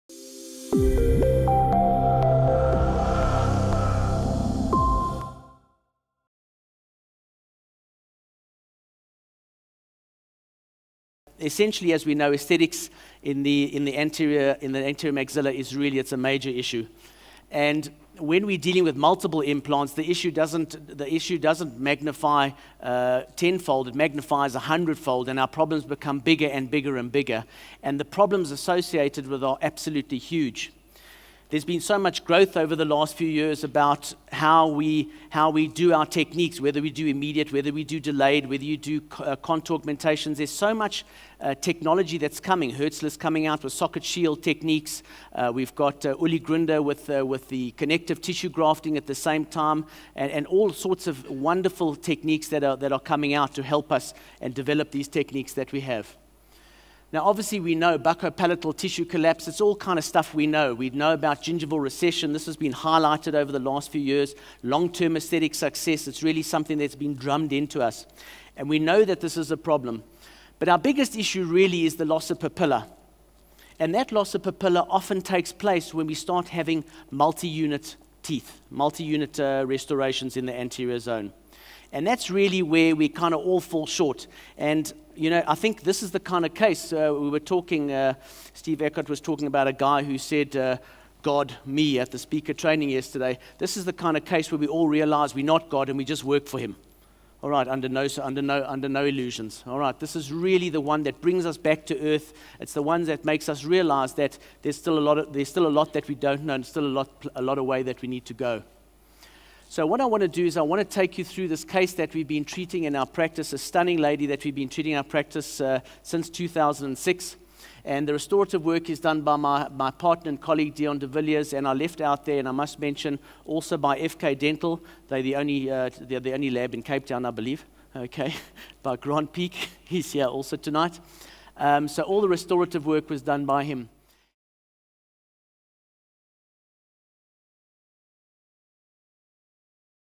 Case Presentation: Root Submergence for Alveolar Bone Preservation | Howard Gluckman
ITI Academy Congress Lecture Preview: Case Presentation: Root Submergence for Alveolar Bone Preservation by Howard Gluckman.

To view the full Congress Lecture, go to the ITI Academy